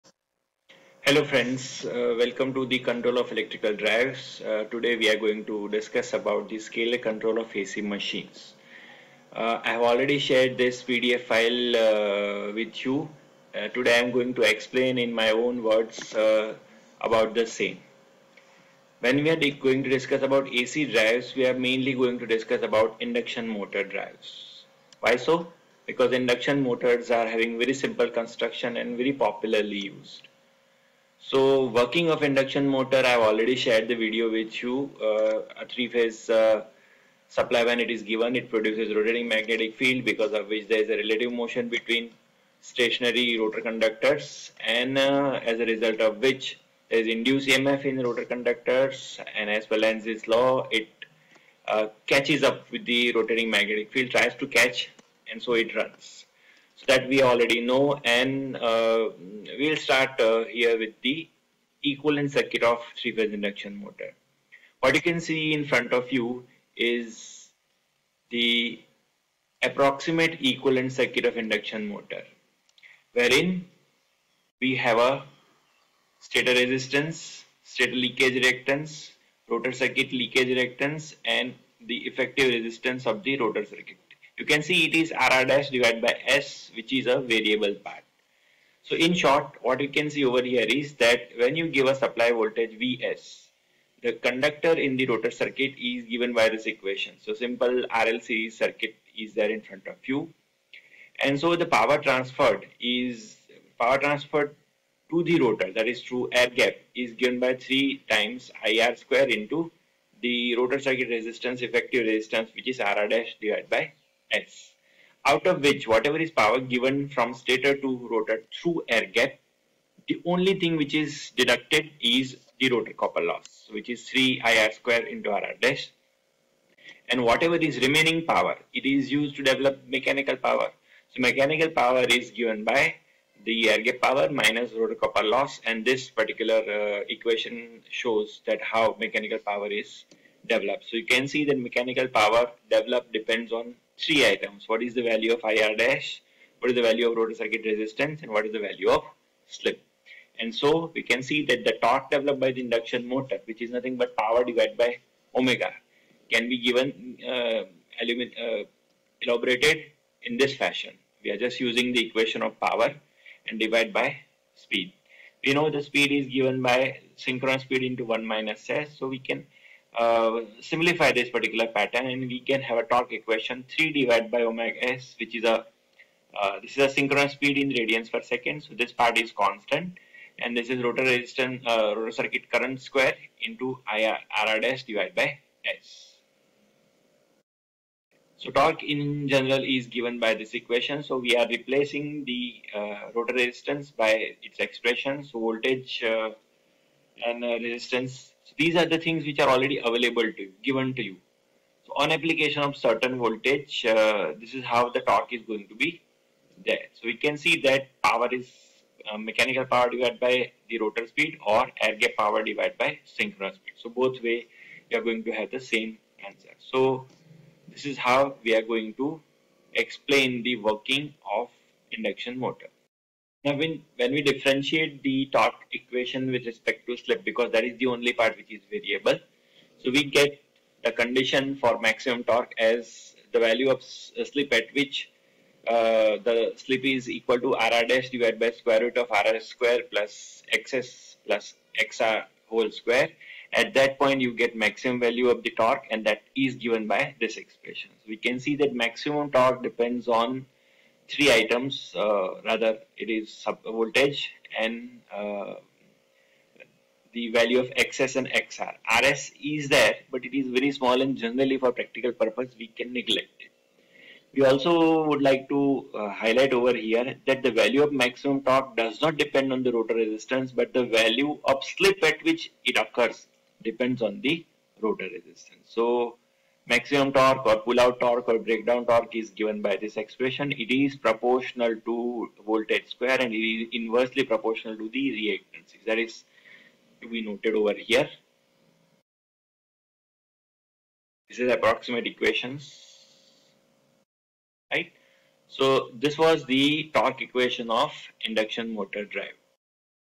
Induction motor torque equation in brief
Before discussing speed control of induction motor drive , it is necessary to quickly revise its torque equation